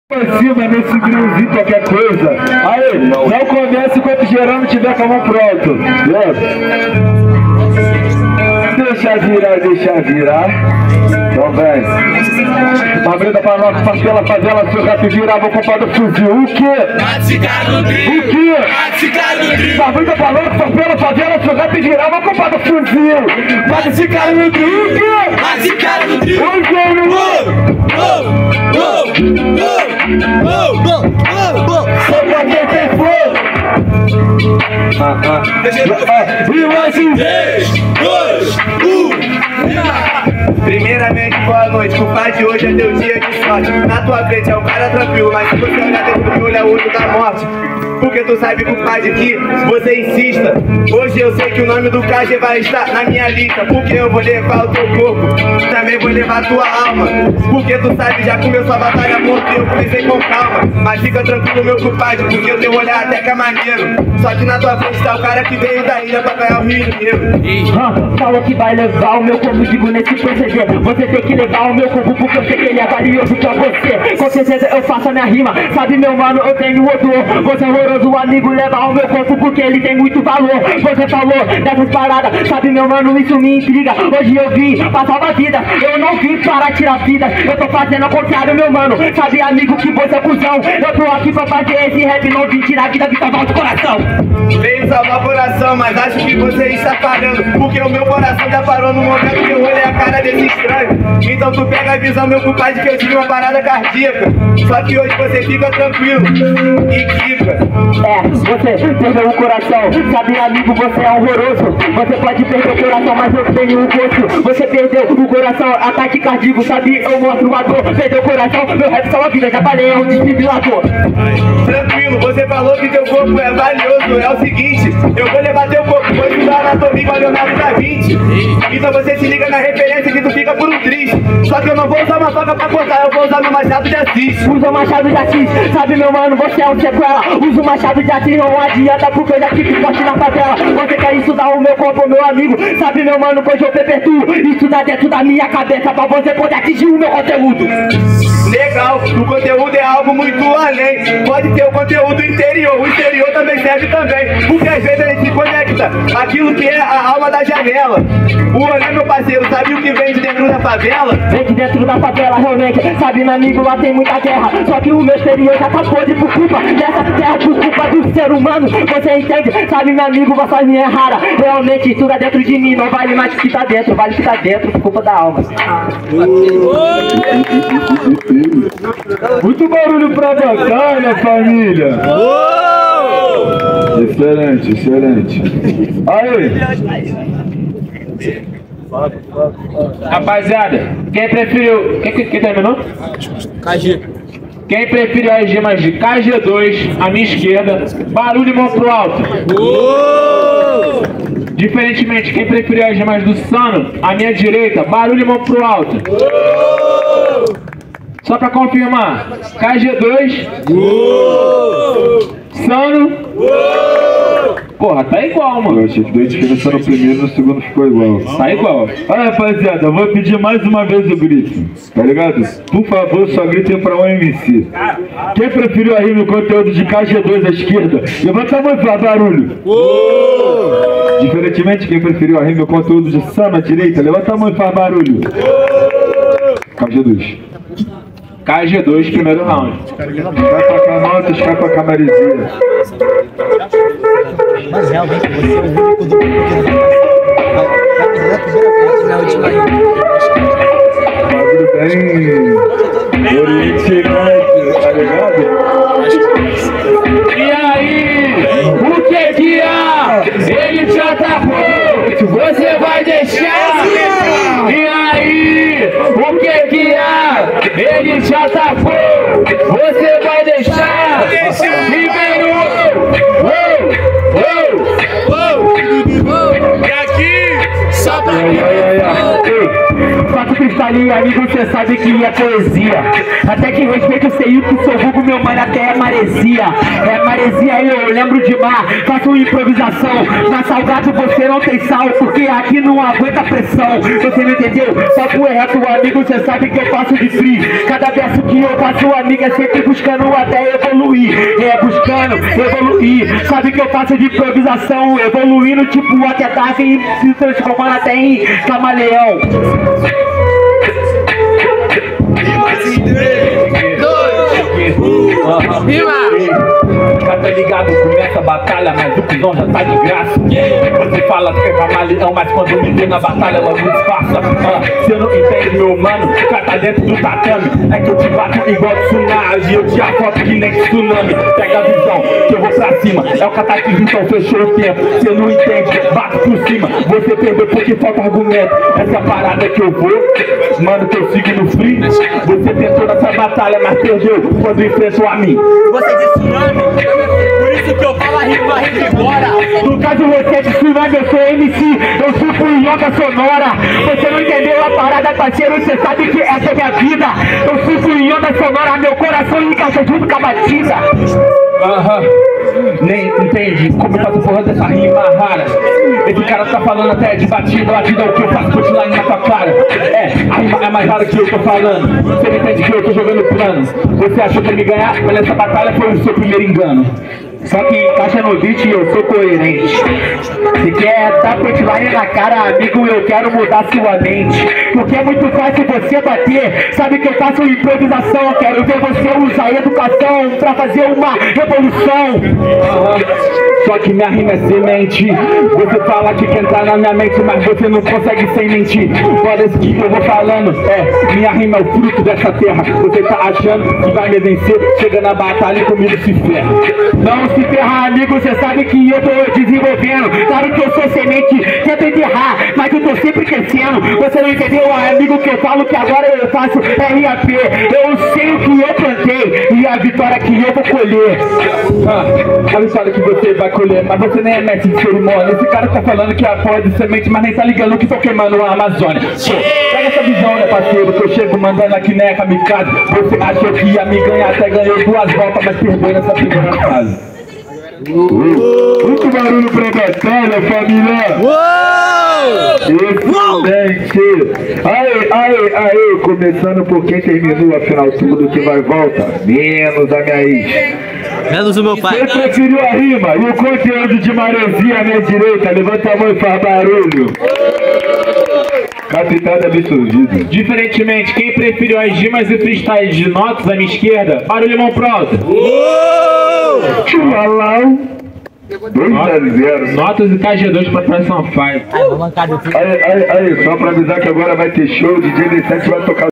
SANO E MAREZIA VS NÓTUS E KG2 PRIMEIRA FASE RODA CULTURAL CANTA TERESA 01/09/2024
a roda cultural canta teresa é realizada no largo do Curvelo, em santa Teresa, a cada quinze dias  a partir das 19:00